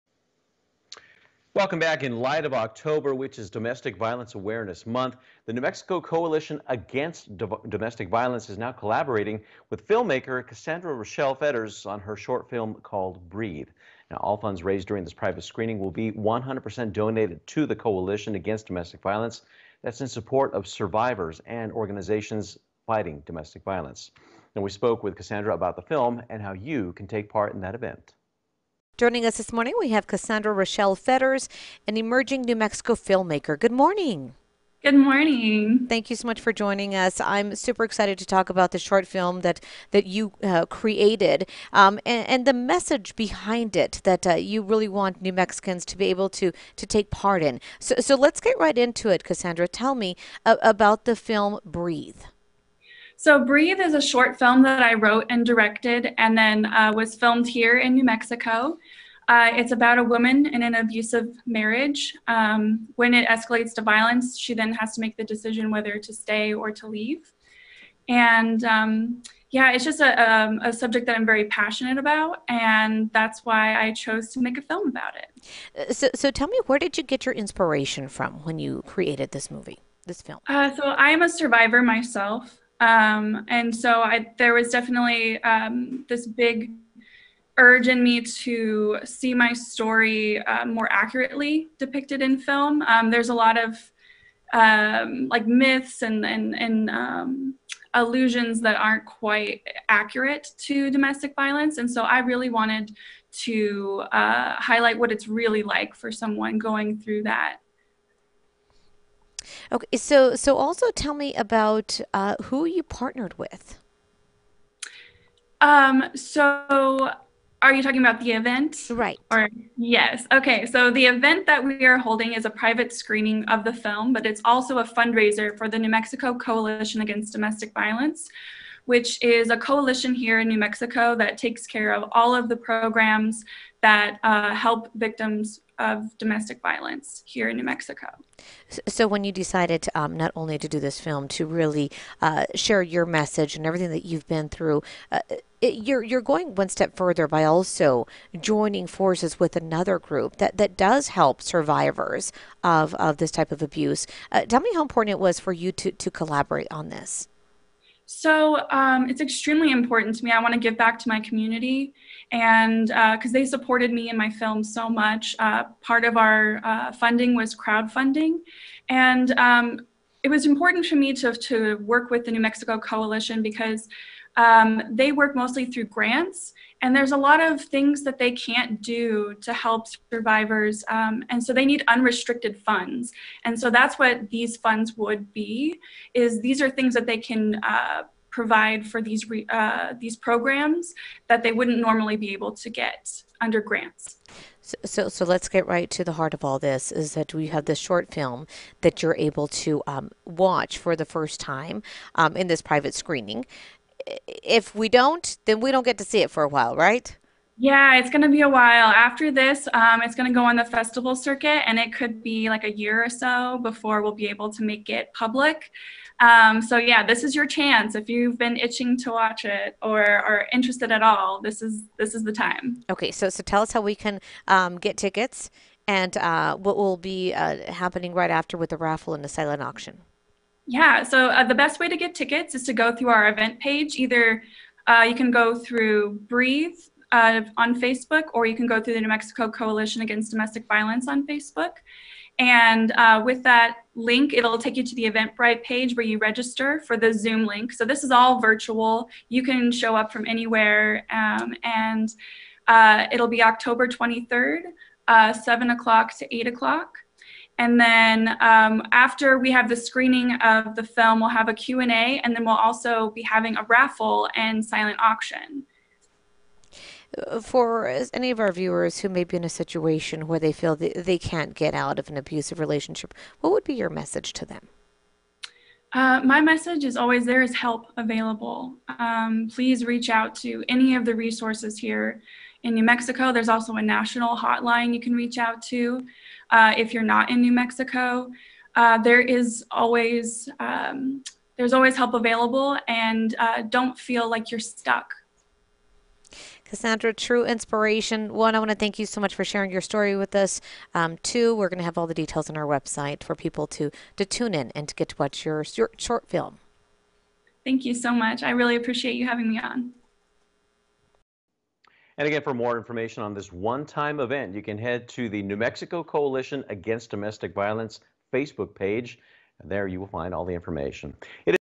Film screening raises funds for New Mexico Coalition Against Domestic Violence
Cassandra Rochelle Fetters discusses her film and how you can take part in this private screening.